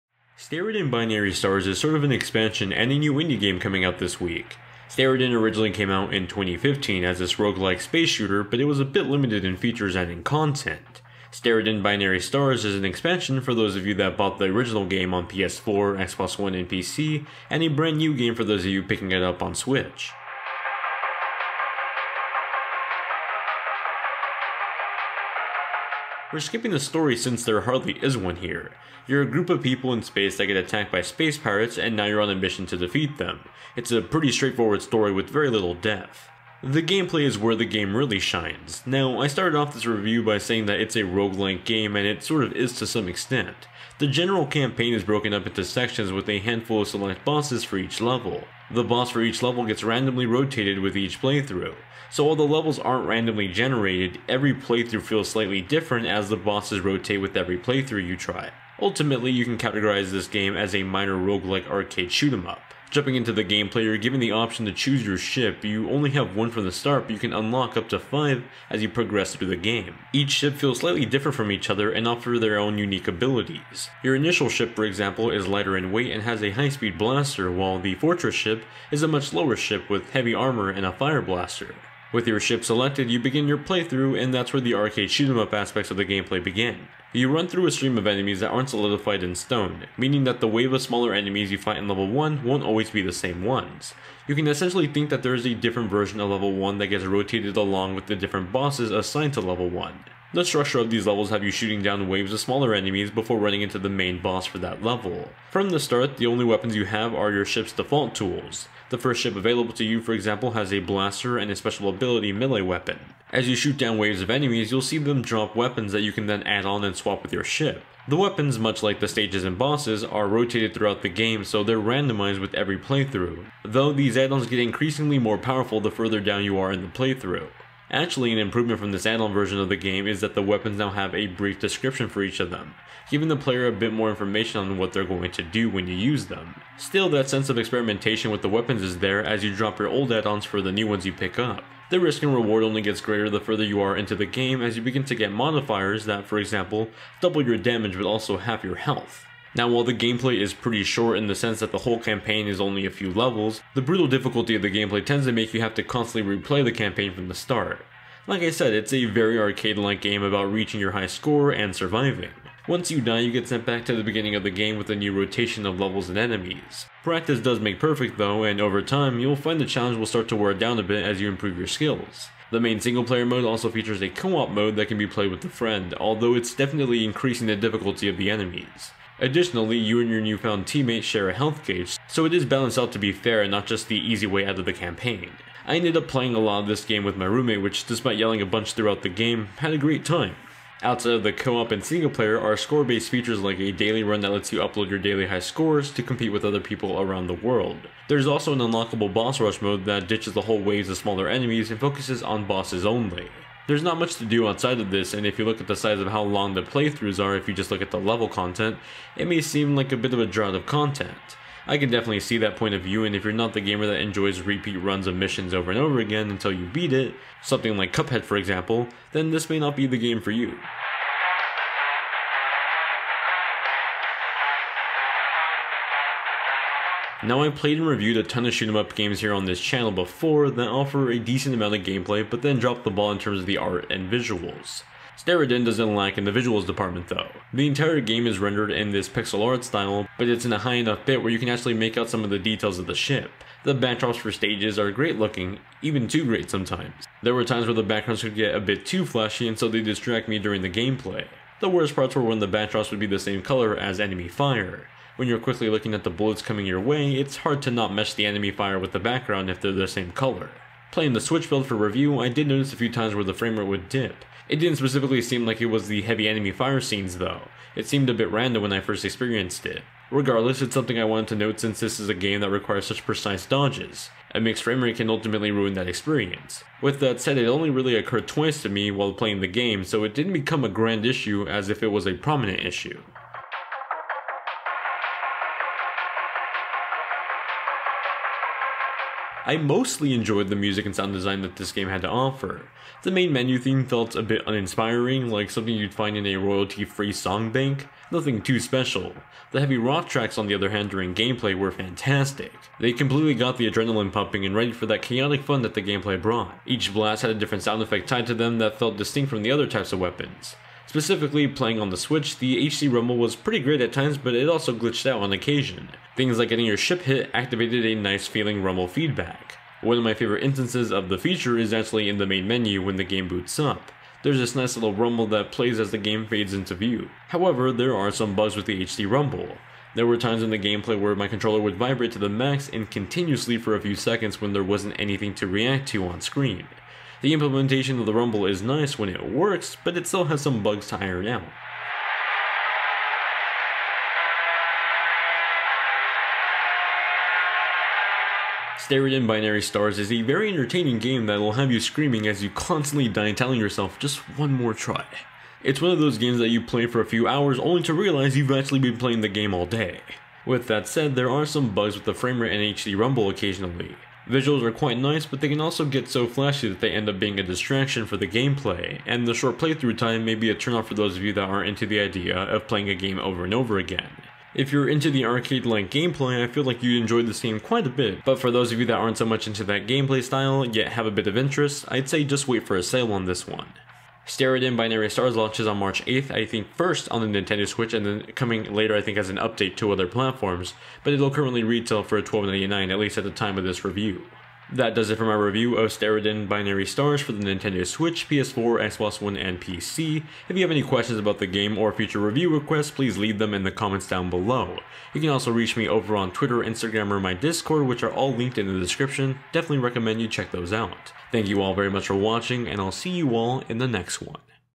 Steredenn: Binary Stars Review
Steredenn: Binary Stars is sort of an expansion and a new indie game coming out. Steredenn originally came out in 2015 as this roguelike space shooter but it was a bit limited in features and content. Steredenn: Binary Stars is an expansion for people that own the original game PS4, Xbox One and PC and a brand new game coming to Switch first.

➜Gaming Usernames:
Wii U: Luis-GVN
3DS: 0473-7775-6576
PS4/PS3/Vita: Luismacintosh
Xbox One: Luismacintosh
Steam: Luismacintosh
BattleNet: LuisGVN#1705